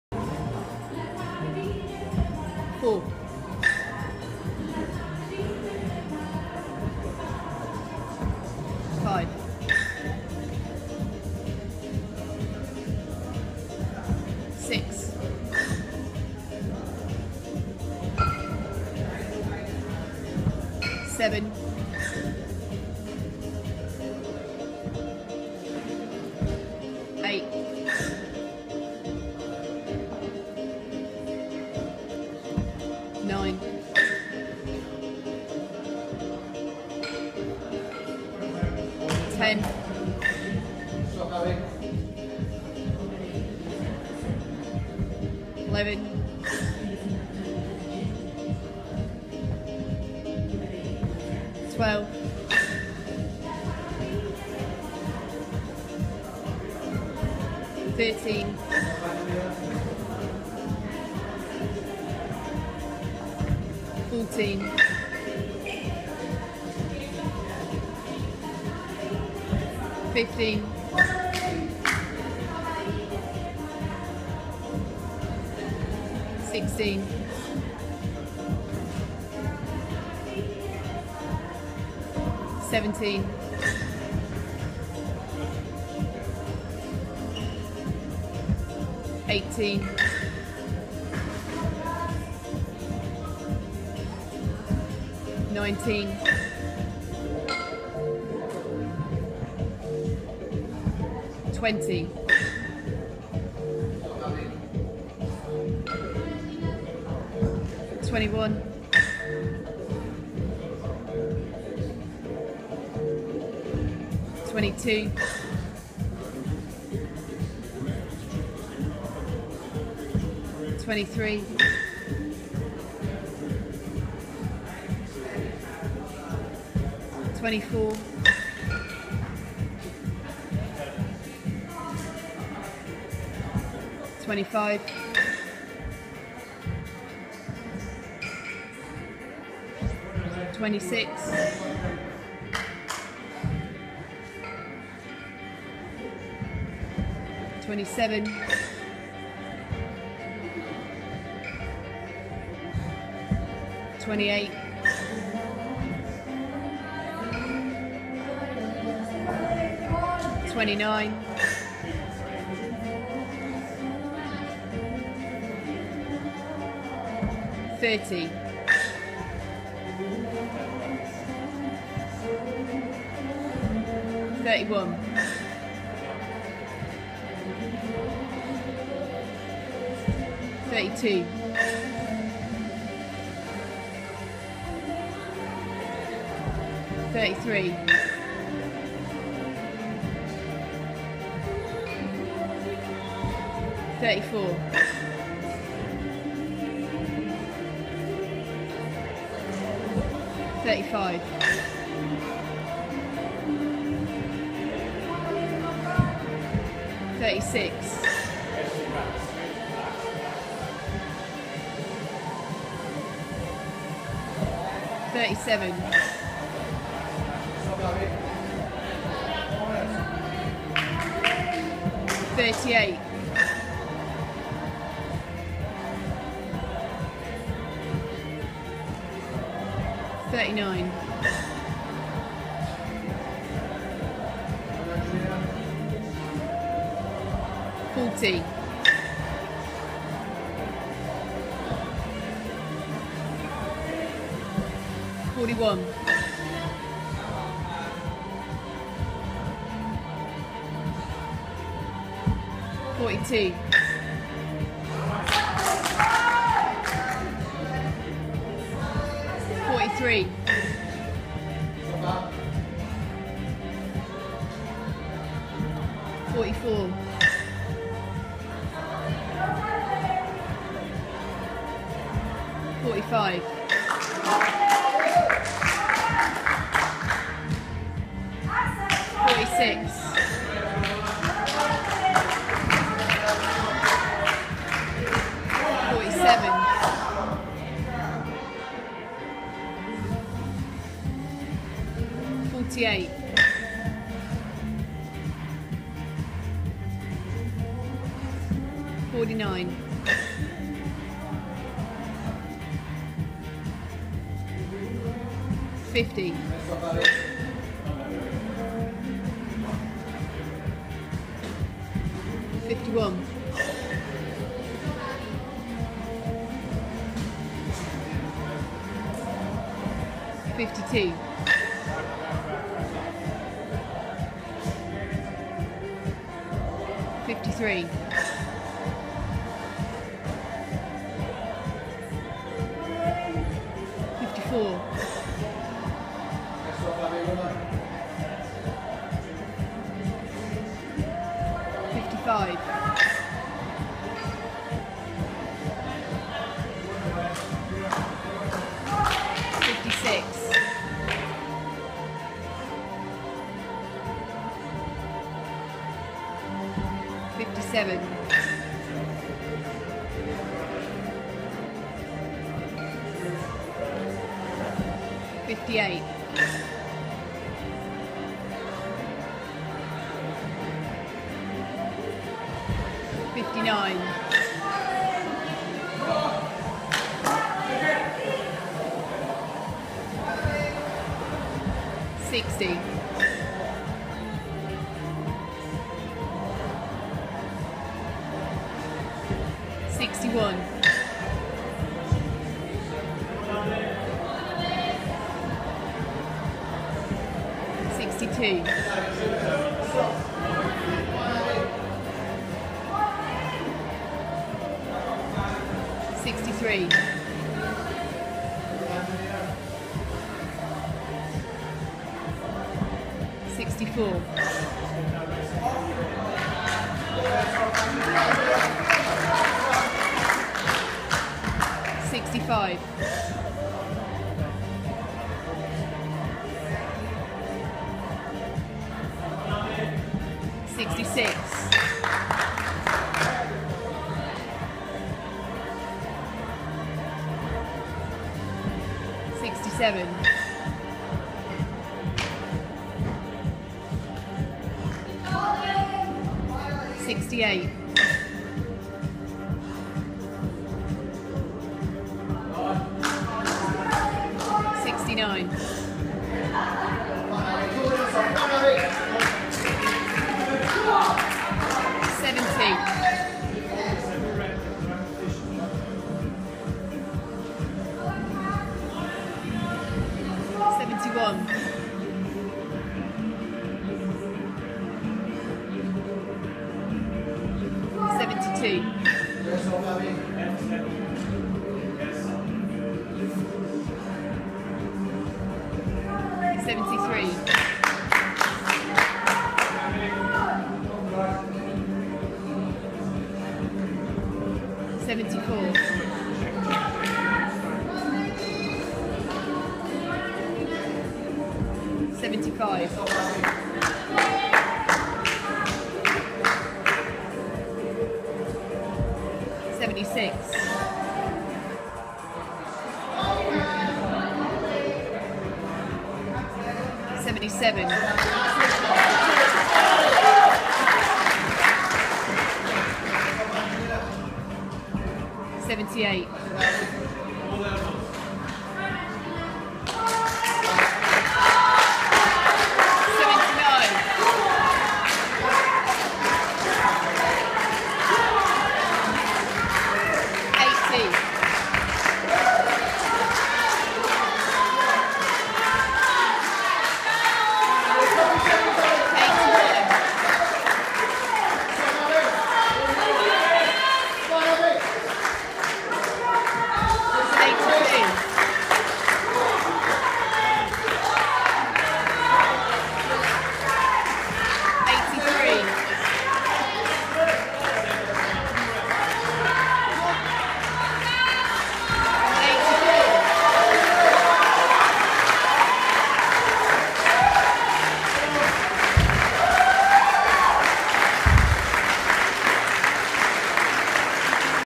Abi Johnston jerk 2x20kg 84reps
Scottish championship 2016
Aberdeen